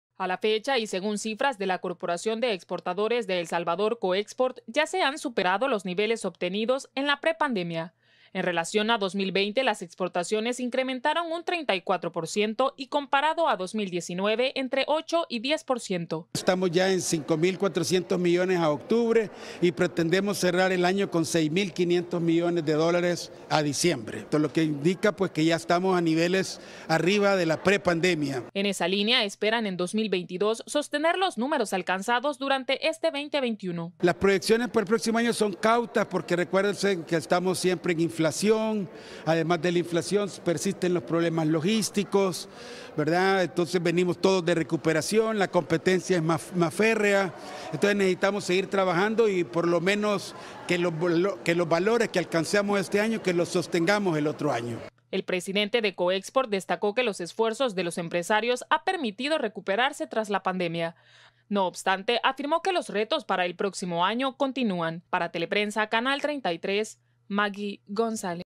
Teleprensa33 | COEXPORT: exportaciones incrementaron 34% en 2021
La Corporación de Exportadores de El Salvador, COEXPORT espera cerrar el año 2021 con 6 mil 500 millones de dólares en envío de mercancías a otras naciones.